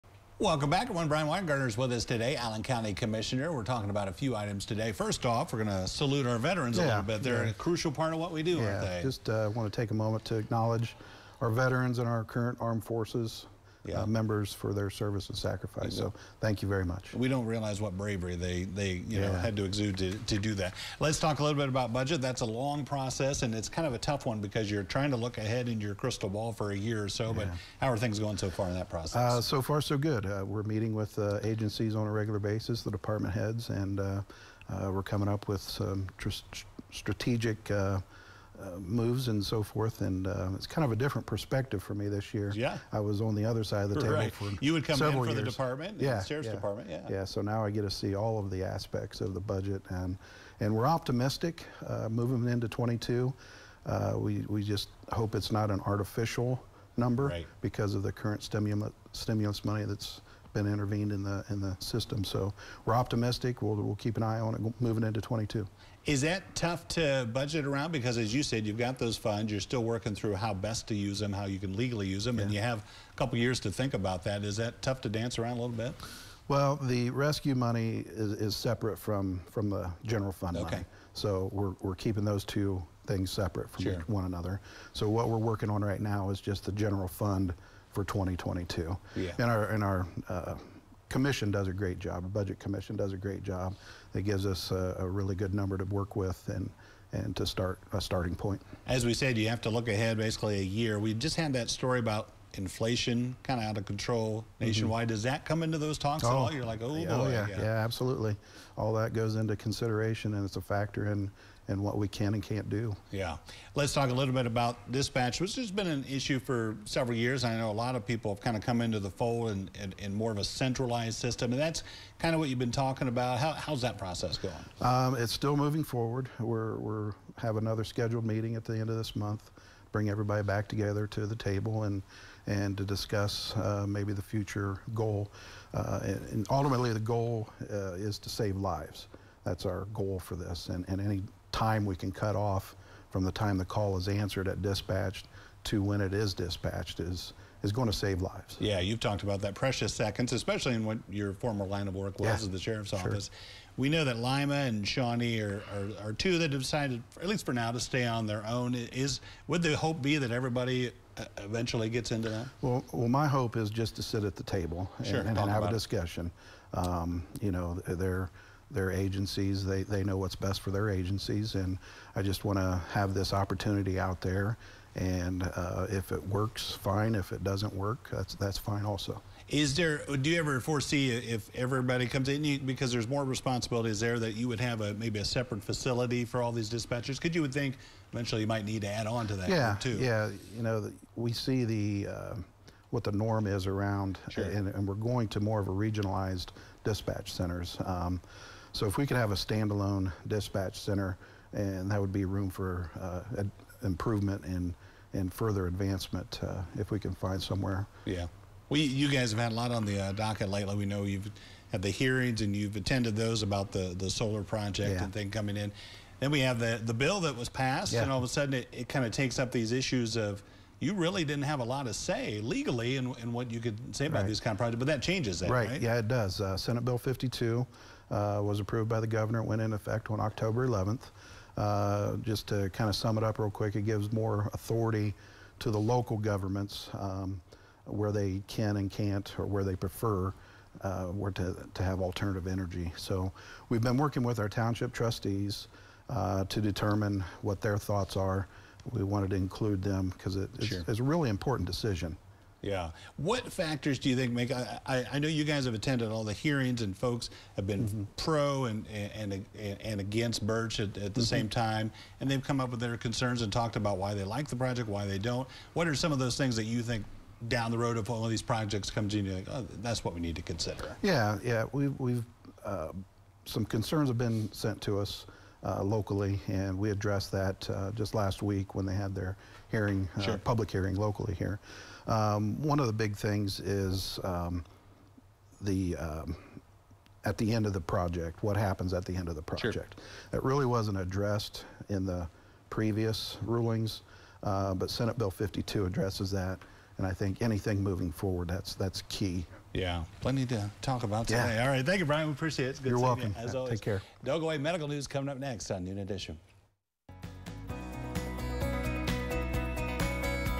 Allen County Commissioners
Commissioner Brian Winegardner talks about budget process, dispatch center and more.